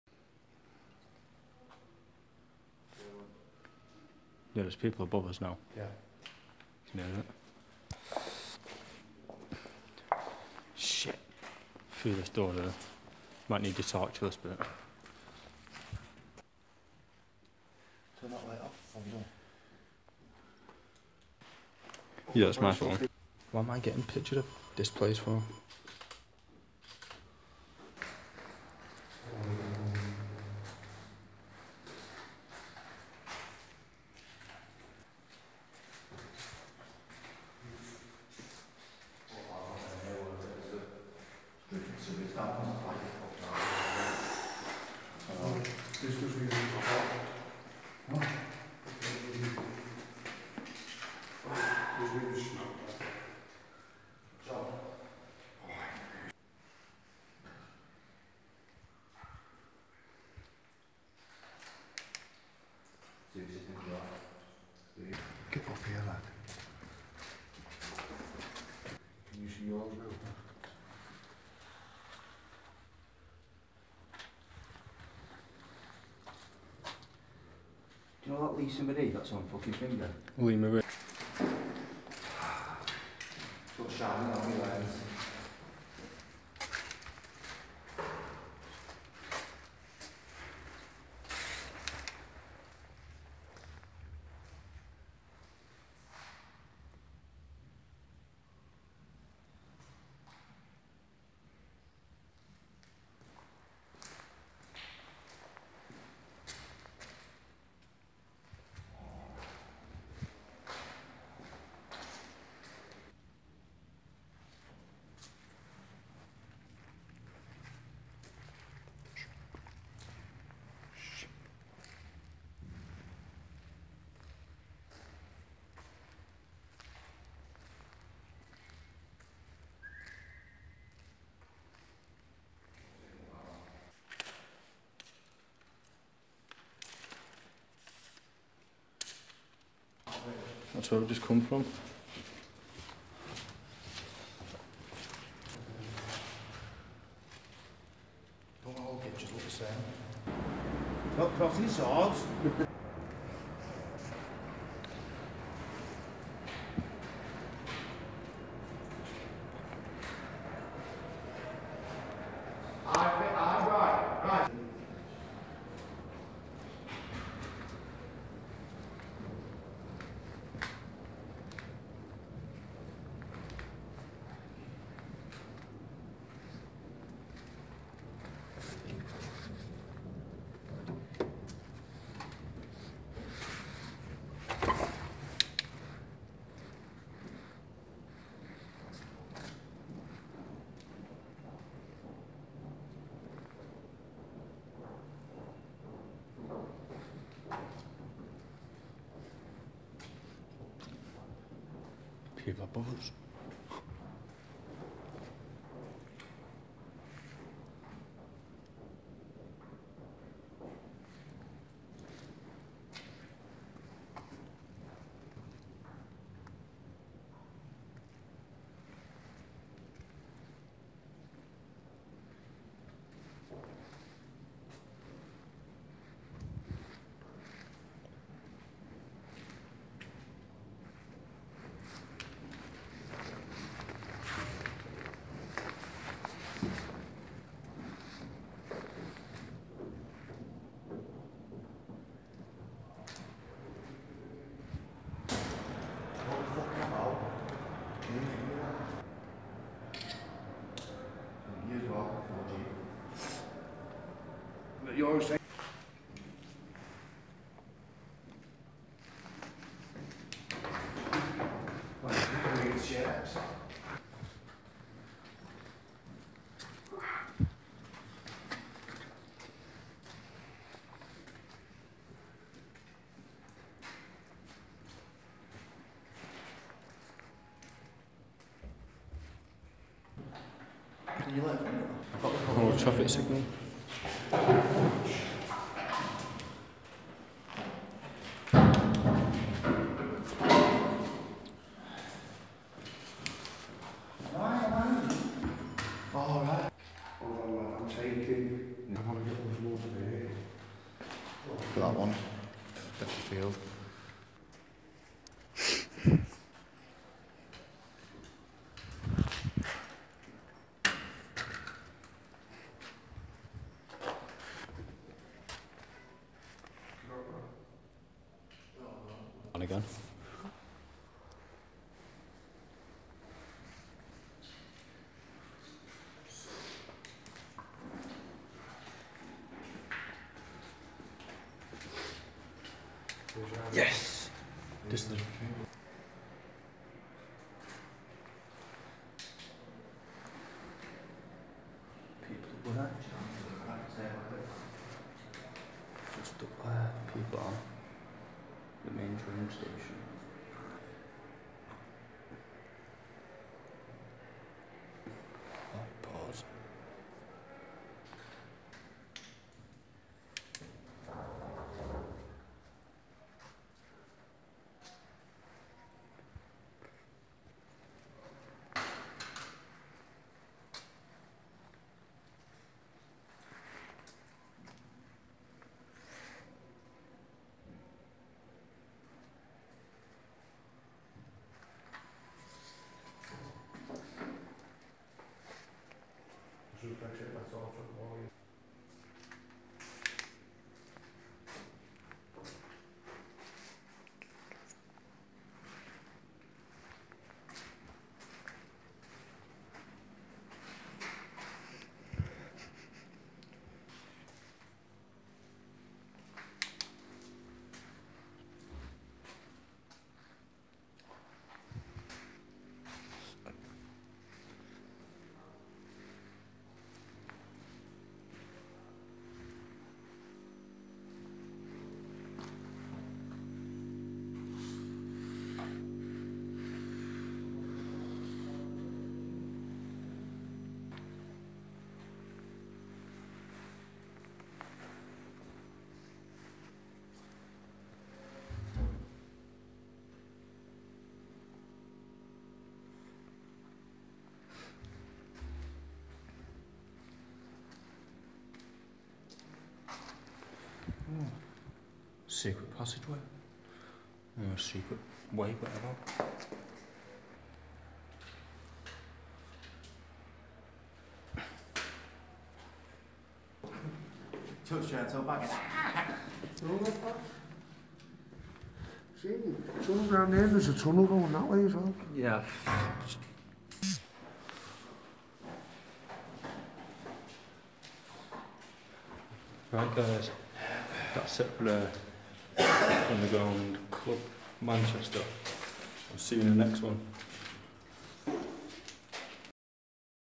Abandoned brsa underground club manchester
Abandoned Brsa club manchester we could hear the train station above us